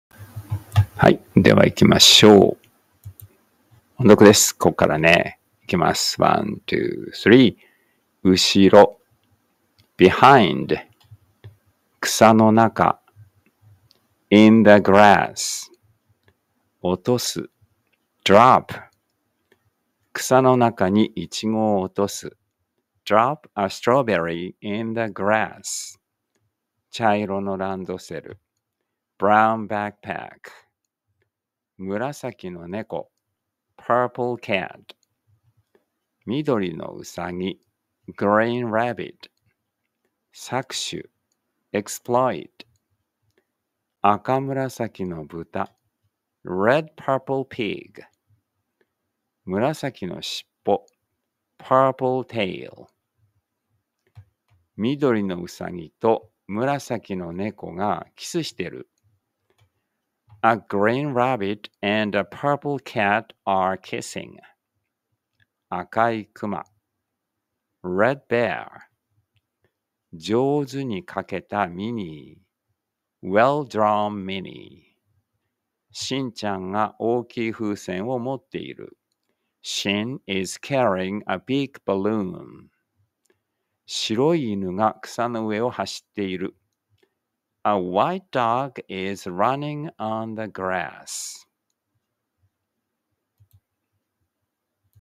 えいご おんどく 99
りんご
apple

うしろ
behind

くさのなか
in the grass

おとす
drop

くさのなかにいちごをおとす
drop a strawberry in the grass

ちゃいろのらんどせる
brown backpack

むらさきのねこ
purple cat

みどりのうさぎ
green rabbit

搾取
exploit

あかむらさきのぶた
red-purple pig

むらさきのしっぽ
purple tail

みどりのうさぎとむらさきのねこがきすしてる。
A green rabbit and a purple cat are kissing.

あかいくま
red bear

じょうずにかけたみにー
well-drawn Minnie

しんちゃんがおおきいふうせんをもっている。
Shin is carrying a big balloon.

しろいいぬがくさのうえをはしっている。
A white dog is running on the grass.